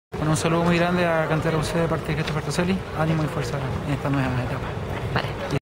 Cristopher Toselli
Saludo del ex canterano, arquero titular del primer equipo, mundialista y seleccionado nacional